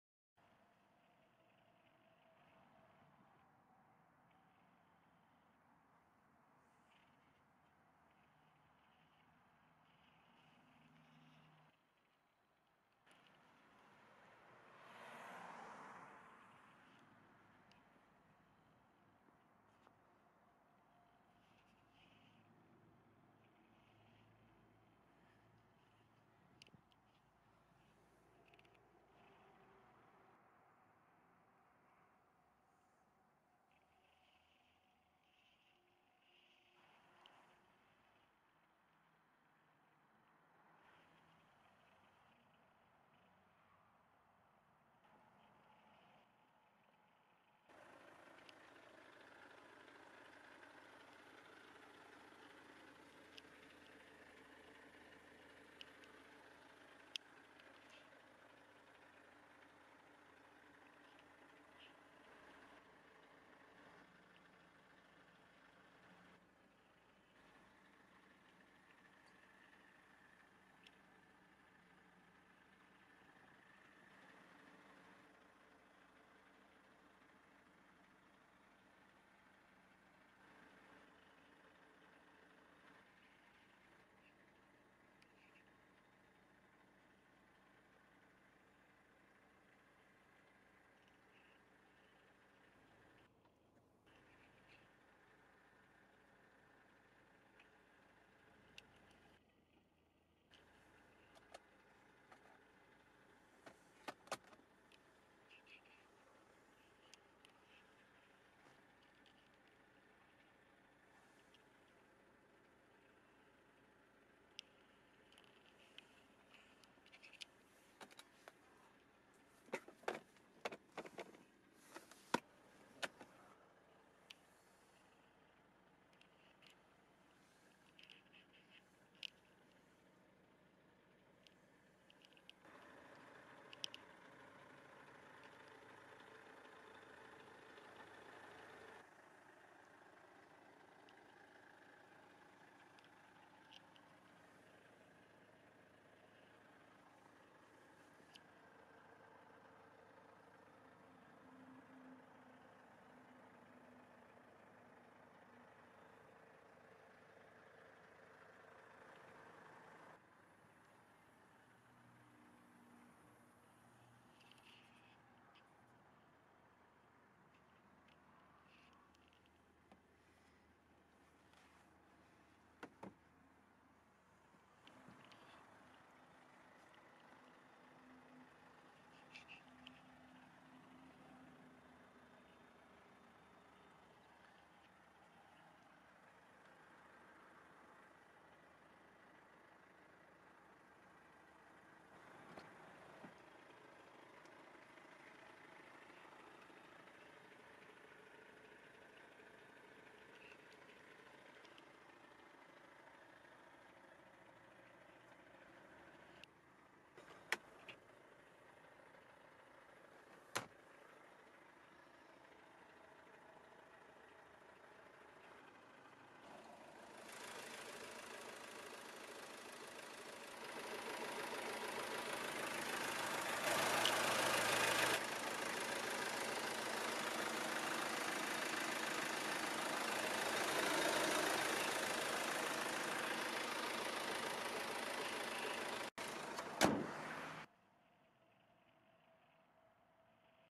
Autokomis Soloch SPRZEDANY: Suzuki Grand Vitara II 1.9 DDIS -Prezentacja. Goczałkowice-Zdrój
Suzuki Grand Vitara II 1.9 DDIS 129KM. Rok 2007. Przebieg: 160 tys km. Cena jak w ogłoszeniu:

SPRZEDANY. Zapraszamy do naszej oferty:

Nasz profil na FB Dźwięk silnika - praca silnika

Autohandel
Car trade
Commerce automobile
Торгівля автомобілями
Торговля автомобилями
تجارة السيارات